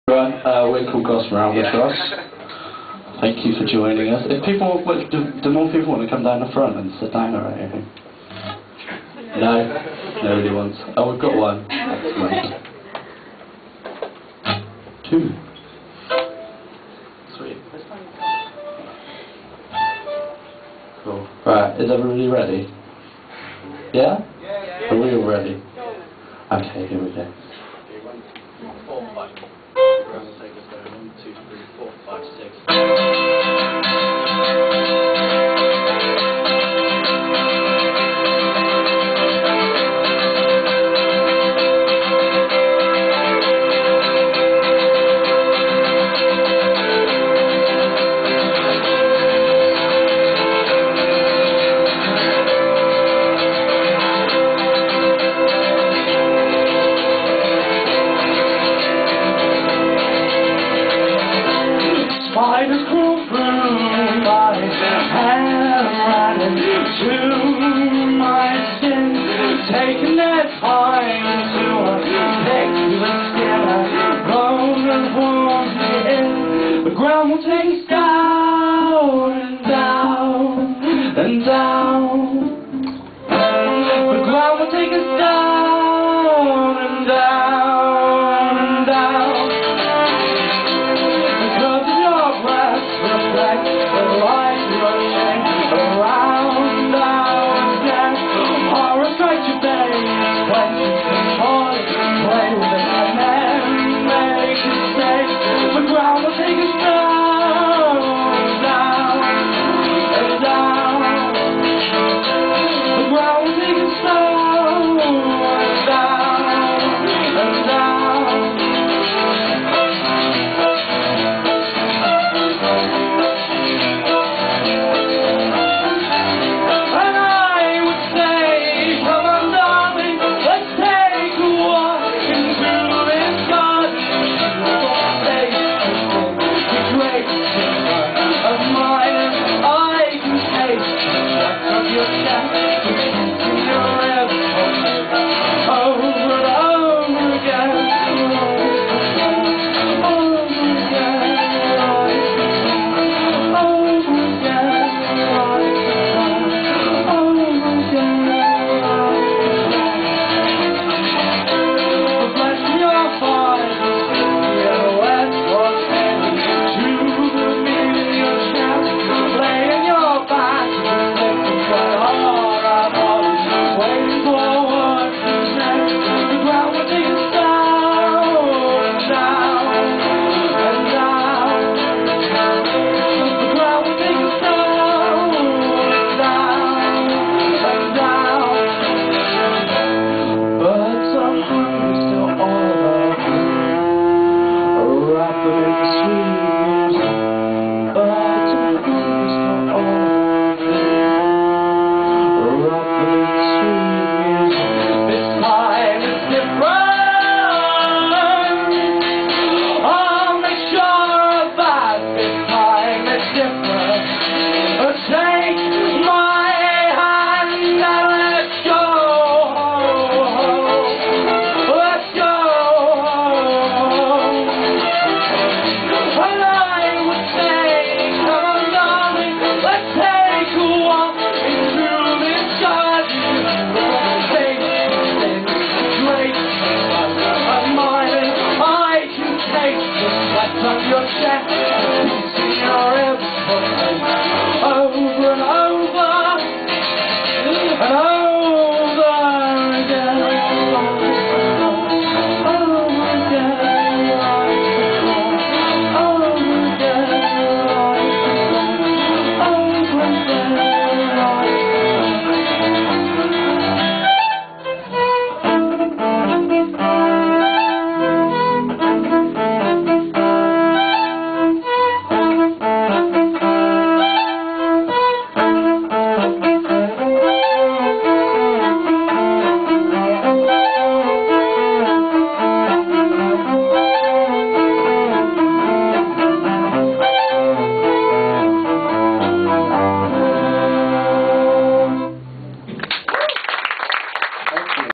Gossamer Albatross (1 of 2) at This Ain't No Picnic Festival, KCLSU, Sep 08
If you are the band or owner of the content in this video and you want me to remove it, please message me I'll delete it straight away.

Gossamer Albatross at This Ain't No Picnic Festival, KCLSU, London. 27 September 2008.